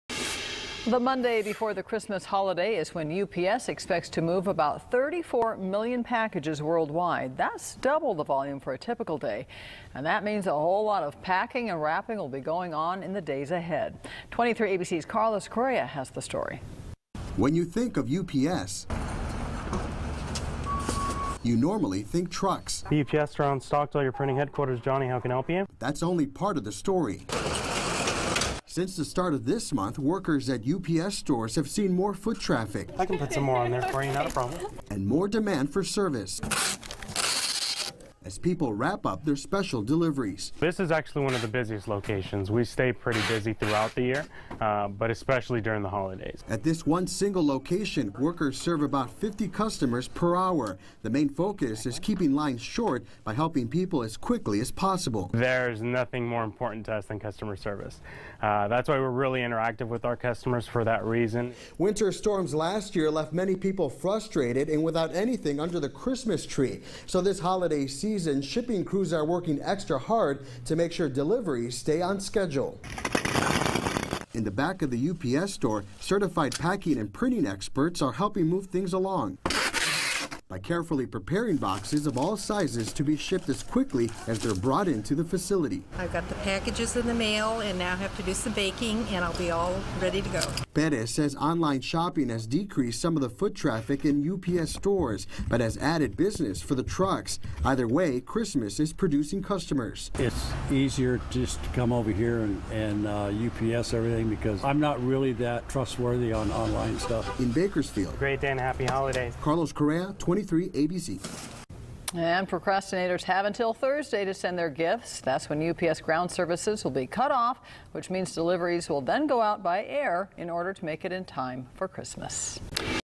UPS prepares for busy shipping day
The Monday before the Christmas holiday is when UPS expects to move about 34-million packages worldwide, that's the double the volume for a typical day.  That means a lot of packing and wrapping will be going on in the days ahead.

When you think of UPS, you normally think trucks, but that's only part of the story.

Since the start of this month, workers at UPS stores have seen more foot traffic and more demand for service as people wrap up their special deliveries.

"This is actually one of the busiest loc